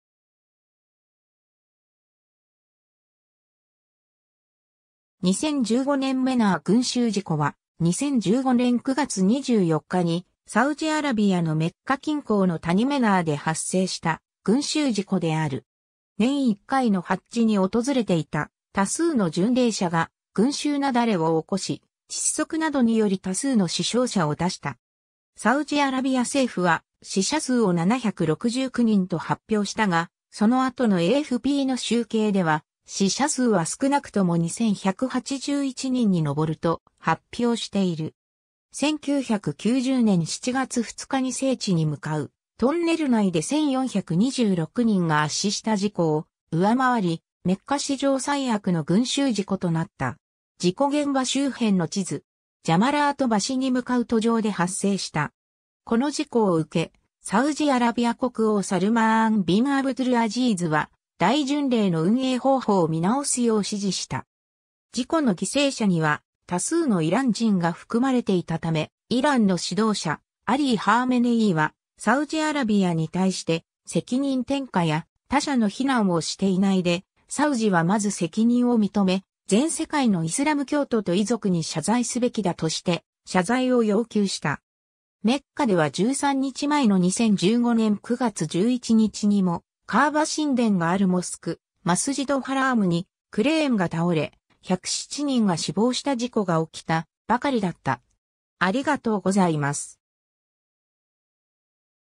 2015年メナー群衆事故
イラン・サウジアラビア関係
2015年メナー群衆事故（2015 Mina stampede）は、2015年9月24日にサウジアラビアのメッカ近郊の谷メナー（ミナ）で発生した群集事故である。
 年1回のハッジ（大巡礼）に訪れていた多数の巡礼者が群衆雪崩を起こし、窒息などにより多数の死傷者を出した。
サウジアラビア政府は死者数を769人と発表したが、その後のAFPの集計では死者数は少なくとも2,181人に上ると発表している。
1990年7月2日に聖地に向かうトンネル内で1,426人が圧死した事故を上回り、メッカ史上最悪の群集事故となった。
 事故現場（赤点）周辺の地図。
ジャマラート橋（地図左上）に向かう途上で発生した。
 この事故を受け、サウジアラビア国王サルマーン・ビン・アブドゥルアズィーズは大巡礼の運営方法を見直すよう指示した。
事故の犠牲者には多数のイラン人が含まれていたため、イランの指導者アリー・ハーメネイーはサウジアラビアに対して「責任転嫁や他者の非難をしていないで、サウジはまず責任を認め、全世界のイスラム教徒と遺族に謝罪すべきだ」として謝罪を要求した。
 メッカでは13日前の2015年9月11日にも、カーバ神殿があるモスク「マスジド・ハラーム」にクレーンが倒れ、107人が死亡した事故が起きたばかりだった。